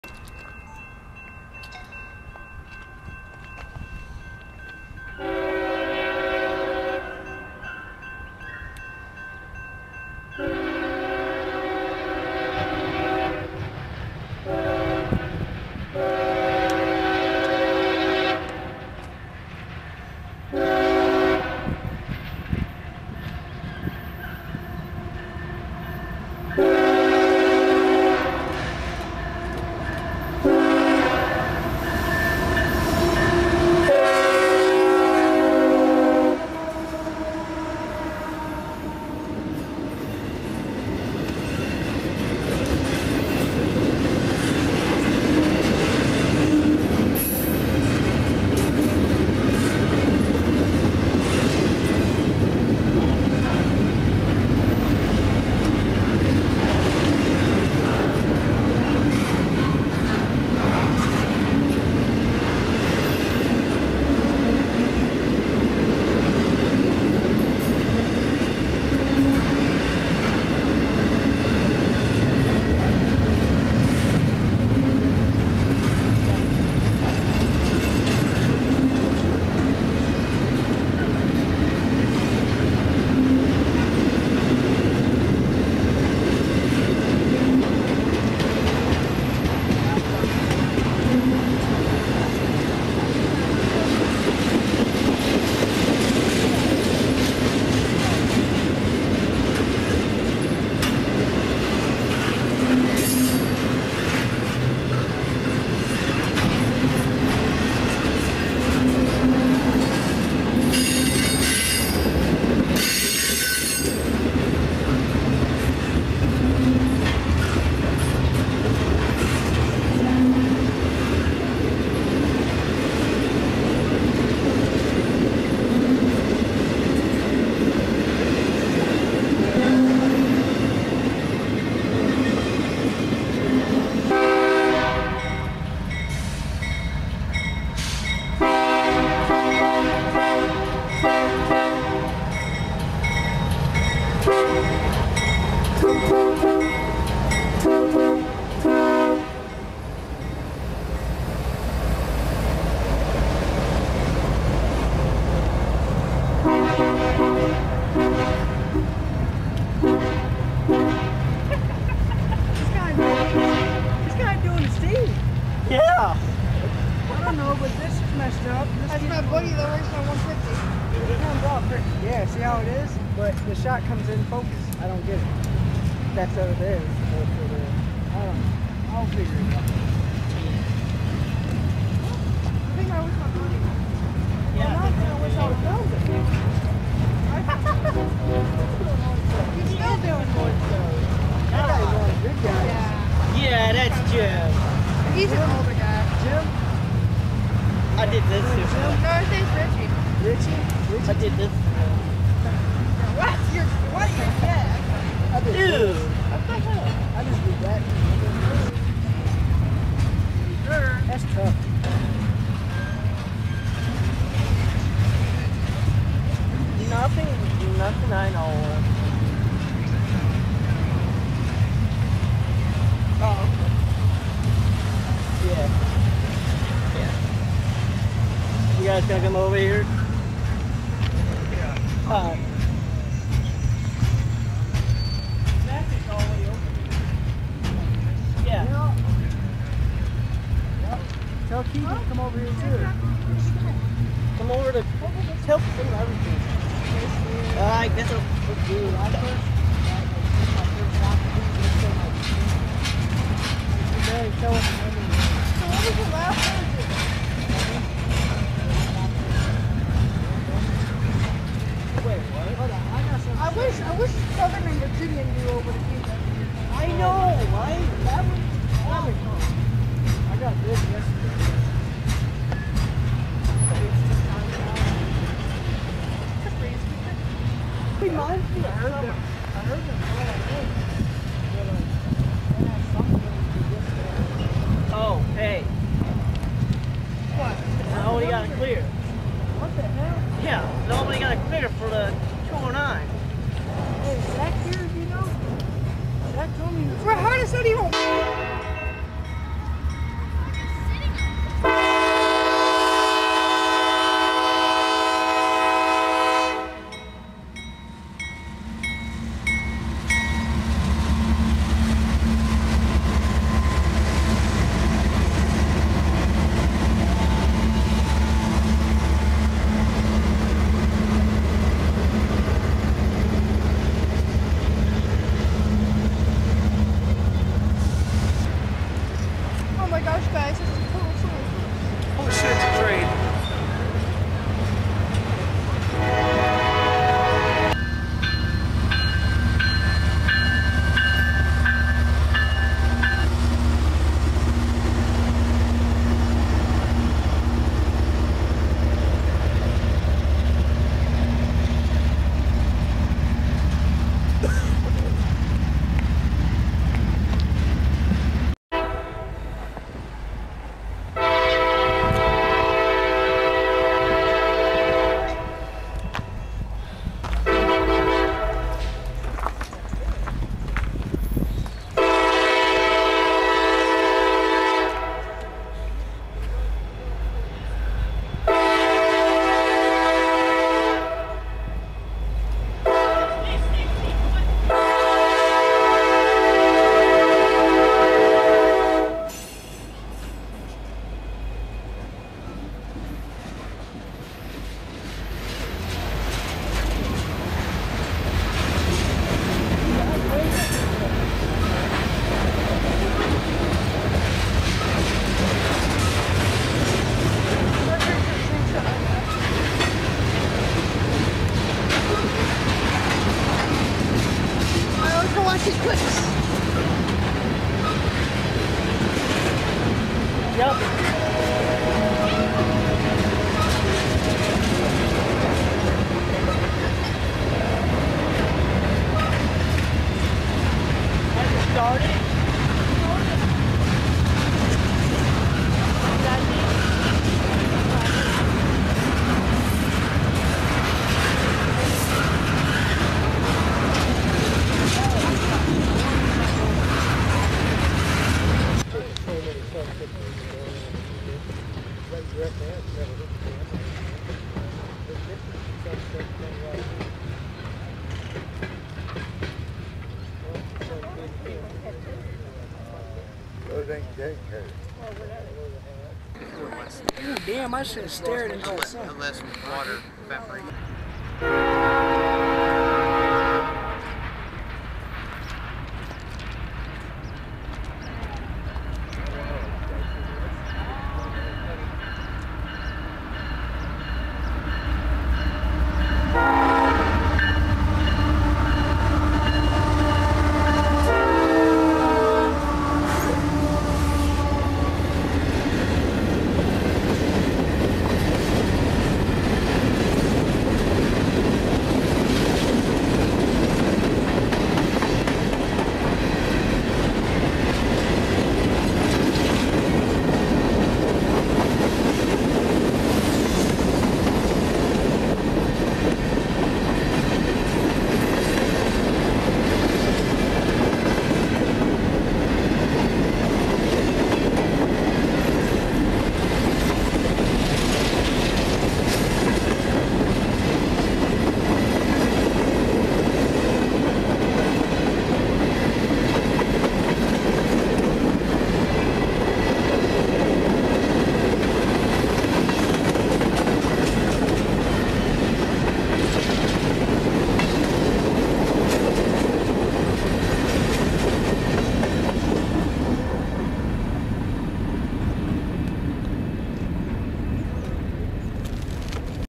Solar Eclipse On VHS, Fostoria Iron Triangle
Welcome Back Today VHS Video that we Take A Look that I would take some VHS Tapes Along The Day At Fostoria It was packed of people and I would do some railfanning before and after The Solar Eclipses